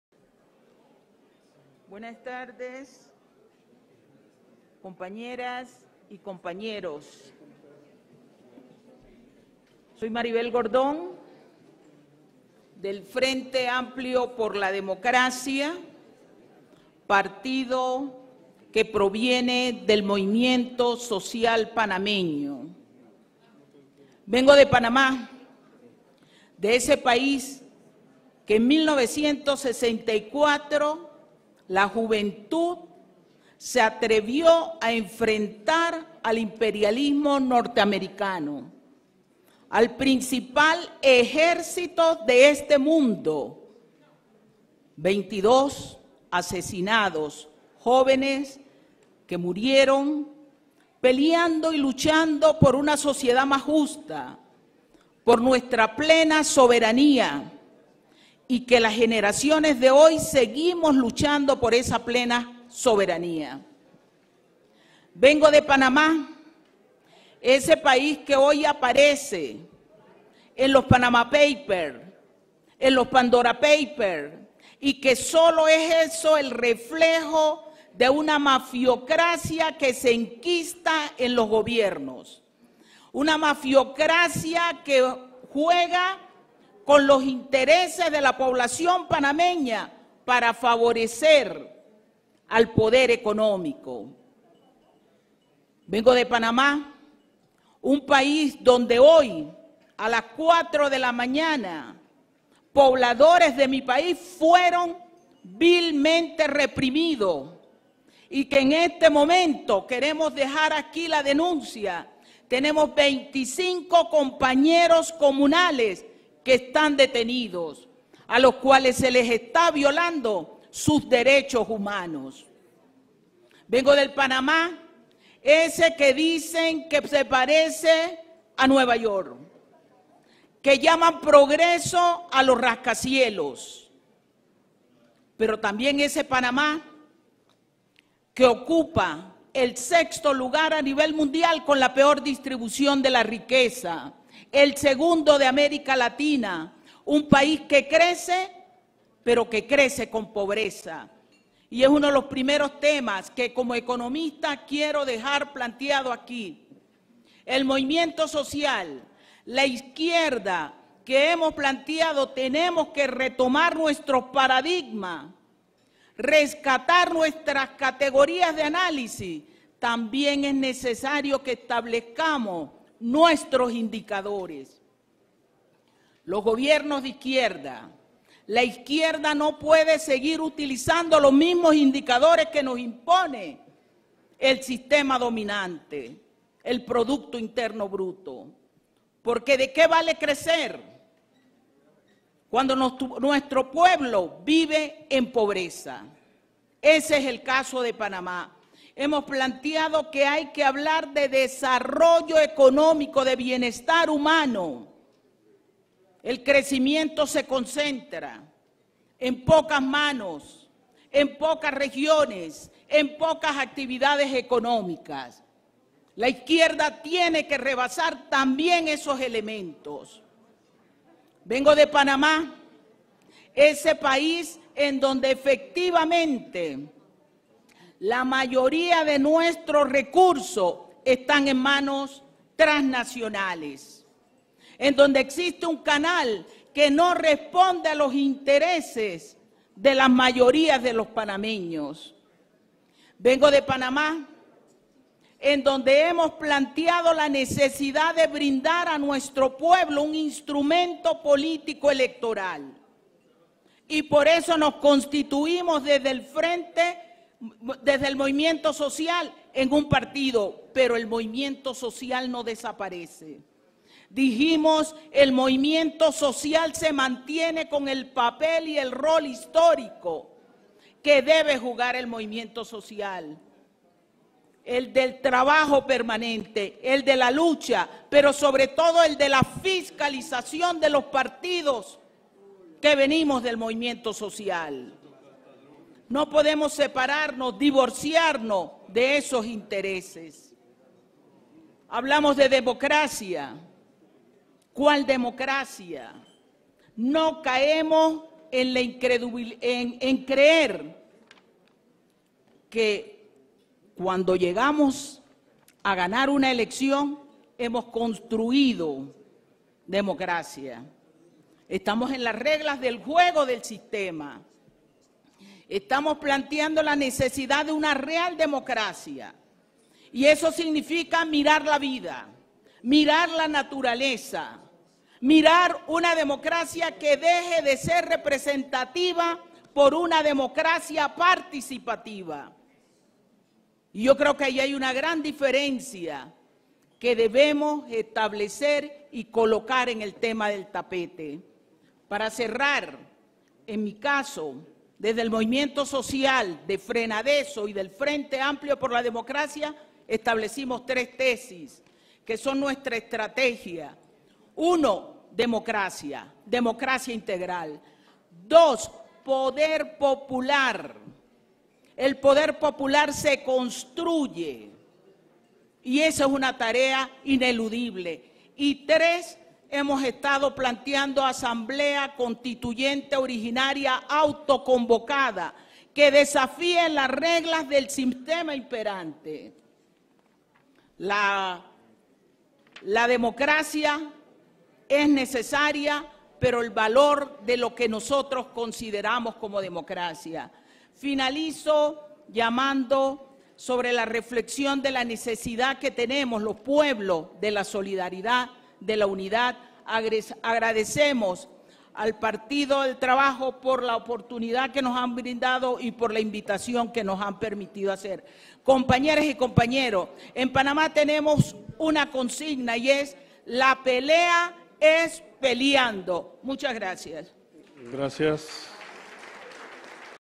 El FAD en el XXV Seminario Internacional del Partido del Trabajo en México 22 10 21
Desde el XXV Seminario Internacional del Partido del Trabajo en México, la vicepresidenta del Frente Amplio por la Democracia, Mgtr, Maribel Gordón.